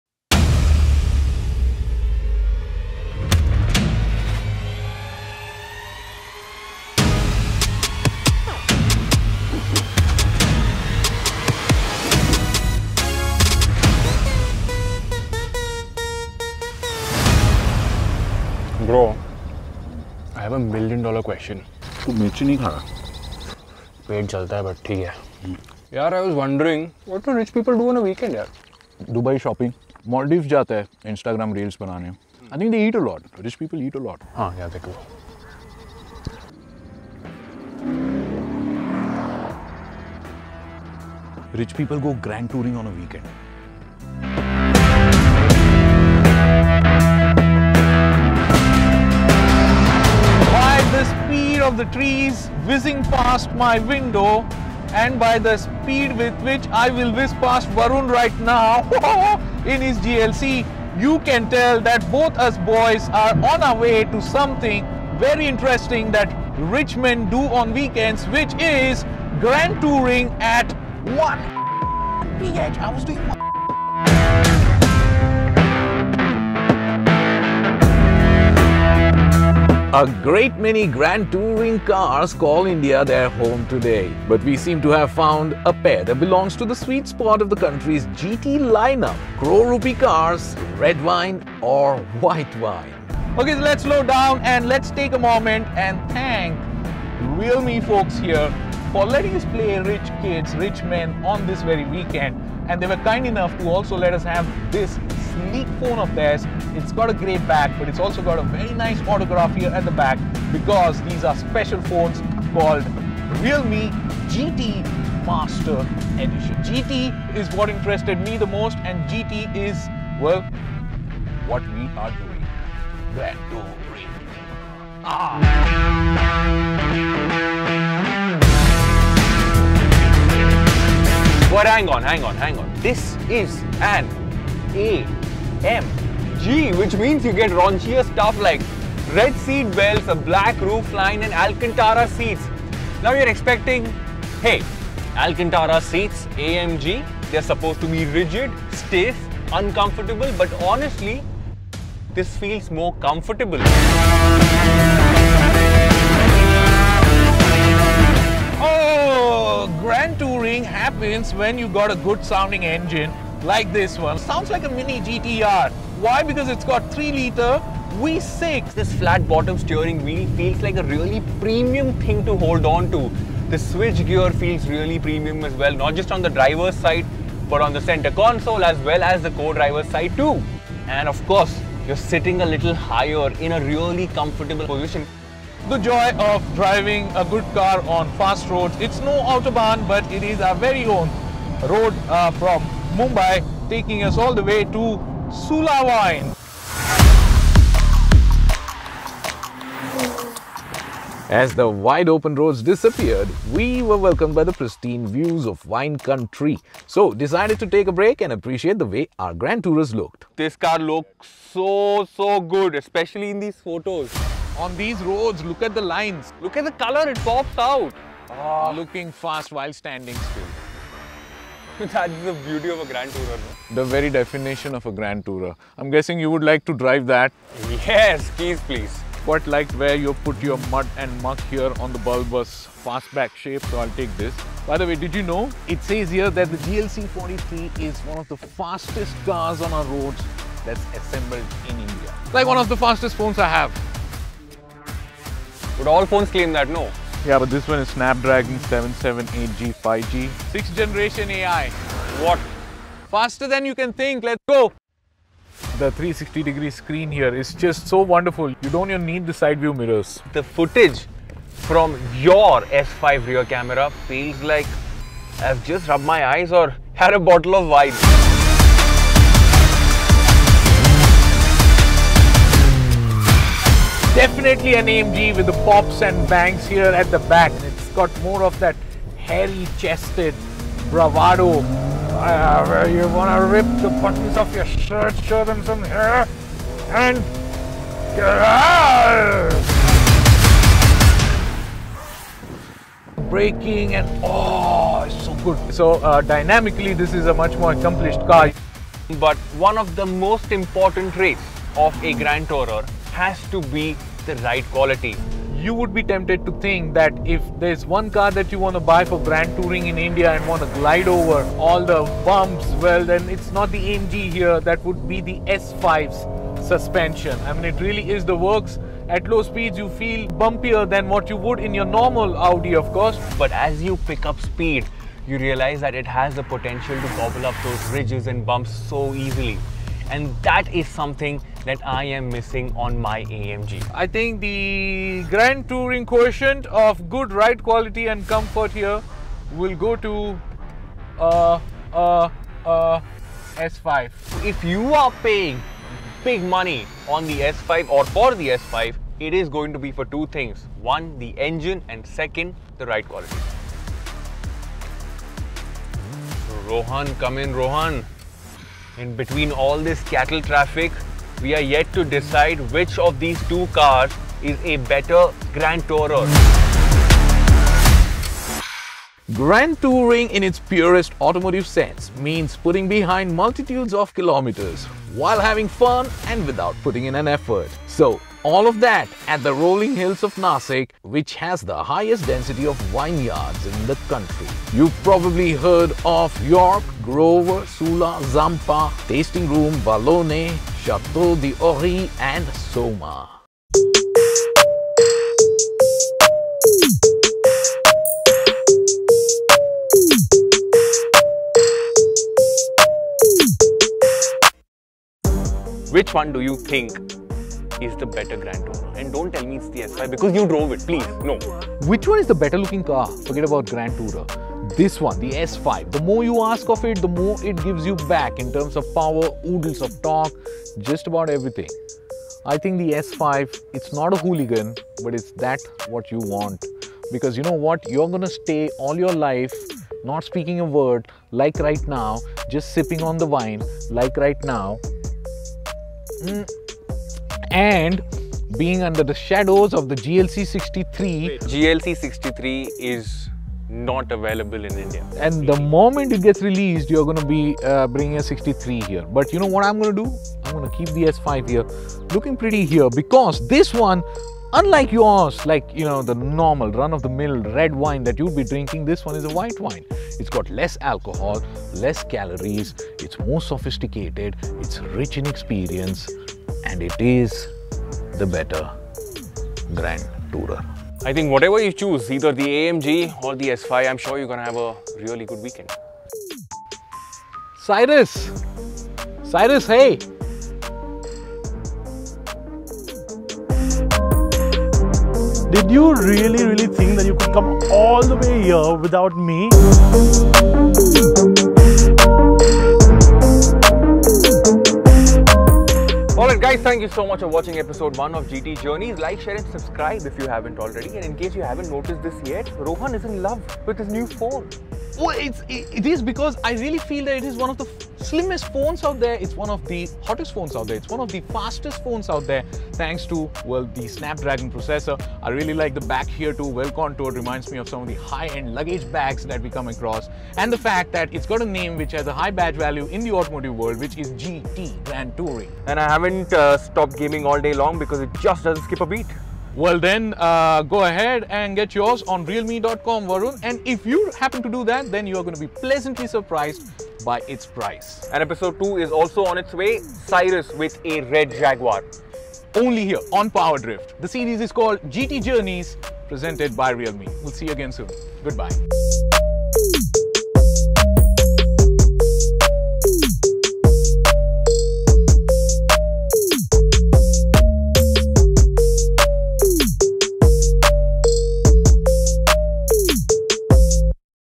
GT Journeys | Episode 1 | PowerDrift x realme India
Welcome to the first episode of GT Journeys powered by realme India featuring realmeGTME Master Edition! Rohan and Varun are on a trip to find out what rich people do on weekends. And for that, they have some of the best GT cars sold in India - the Mercedes-AMG GLC43 and the Audi S5!